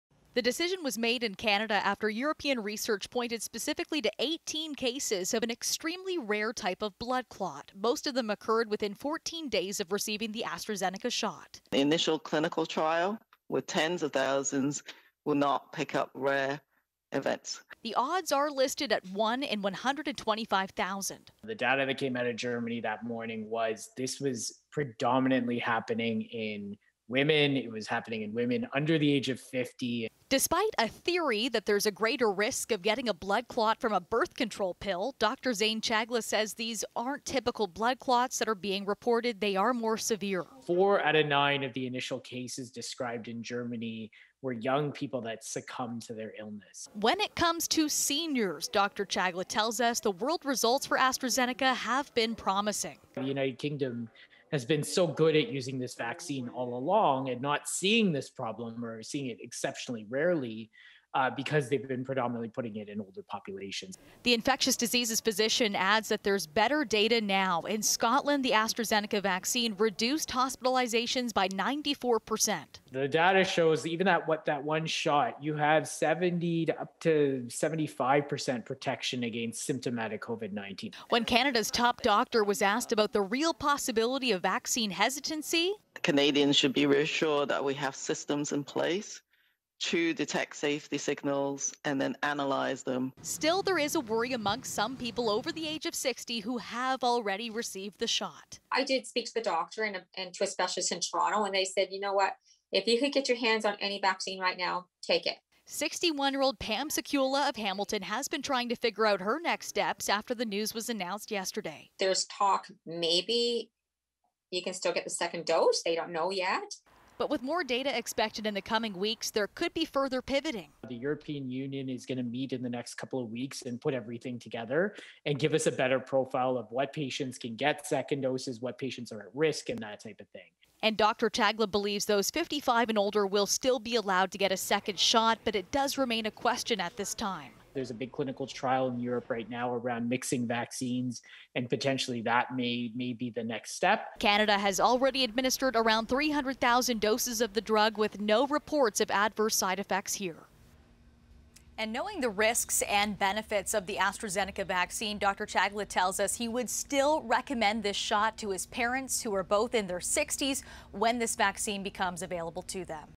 Will renewed concerns over the AstraZeneca vaccine stop people from getting the shot?
Concern about the AstraZeneca vaccine and blood clots has pushed all provinces to stop using it for people under 55. Nicole Martin spoke with an infectious diseases physician about the findings in Europe that prompted the pause and what this means for those 60 and older who already received the shot.